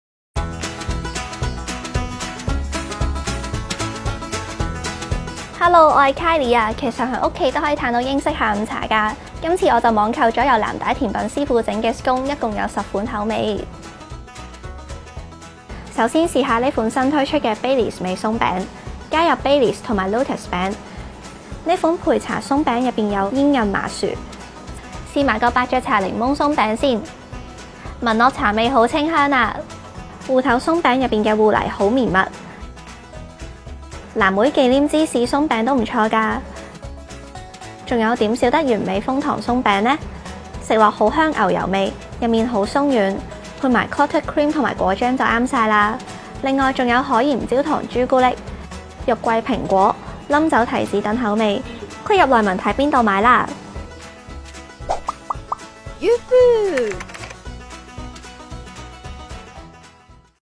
【UFood開箱】藍帶甜品師傅主理新派英式鬆餅網店 Baileys／焙茶麻糬／芋泥／藍莓忌廉芝士Scone
呢間小店嘅Scone有10款口味揀，比起傳統英式鬆餅食落更外脆內軟，好香牛油味！